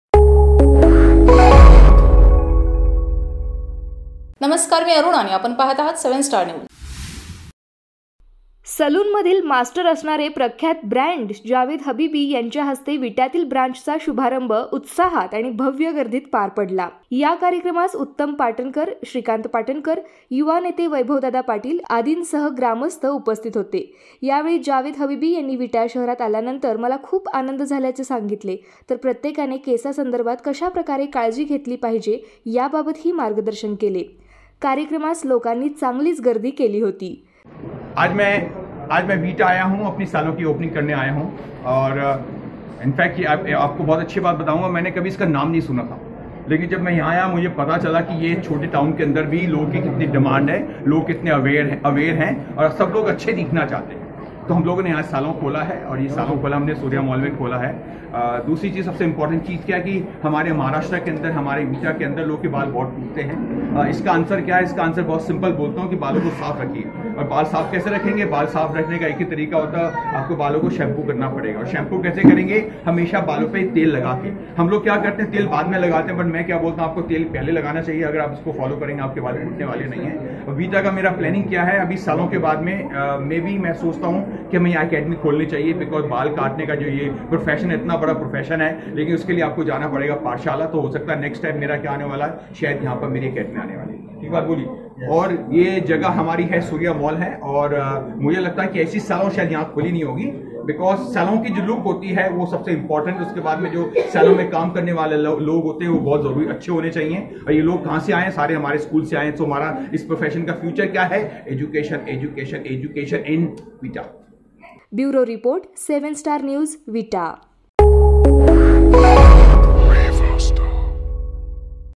विट्यात जावेद हबीबी यांच्या शुभहस्ते सेलॉनचा भव्यदिव्य शुभारंभ संपन्न | 7 STAR NEWS
विट्यात जावेद हबीबी यांच्या शुभहस्ते सेलॉनचा भव्यदिव्य शुभारंभ संपन्न